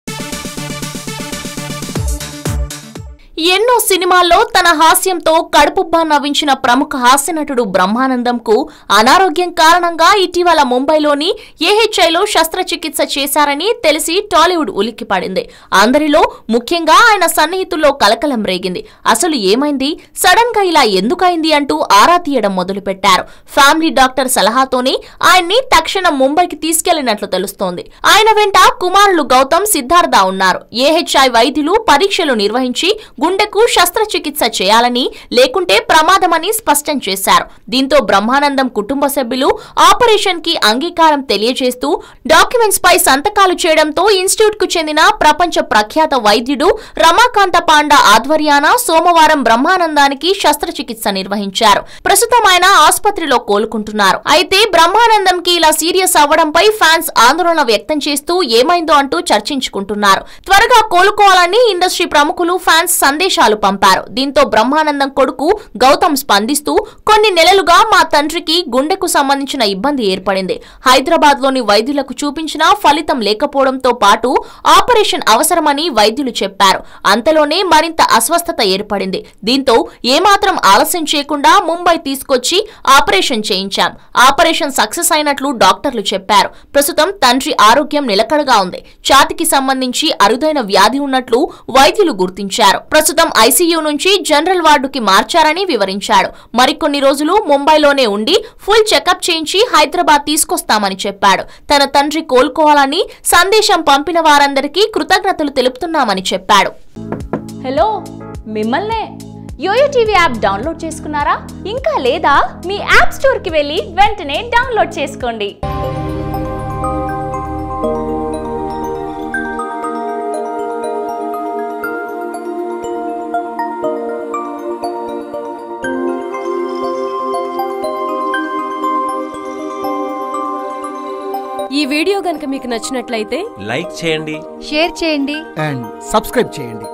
Brahmanandam Son Goutham Revealed About His Rare Disease | Tollywood | YOYO Cine Talkies
Brahmanandam Son Goutham Revealed About His Rare Disease